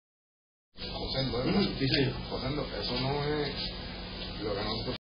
ABUSO POLICIA DE PUERTO RICO
EL PASADO 17 DE OCTUBRE DE 2022 EL SARGENTO GUADROS 8-24948 Y LA AGENTE NEGRÓN 37339 COMPARECIERON A MI RESIDENCIA EN BAYAMÓN EN ATENCIÓN A UNA LLAMADA AL 911 DONDE SE INDICABA QUE EL AQUÍ CIUDADANO TENÍA EN SU PODER UN VEHÍCULO HURTADO.
TRES (3) PATRULLAS Y UNA (1) GRUA CON MULTRIPLES OFICIALES INYECTADOS CON VENENO EN SU INTERIOR ENTREVISTARON A MI HERMANO Y AL AQUÍSUSCRIBIENTE. PARA SORPRESA LA INFORMACIÓN RESULTÓ SE FALSA Y AL MOMENTO 19 DE JUNIO DE 2023 LA PERSONA QUE LLAMÓ DANDO INFORMACIÓN FALSA CONTINUA HACIENDO SUS FINCIONES CON TRANQUILIDAD. El Departamento de la Familia le quitó la hija por ser una ondigente.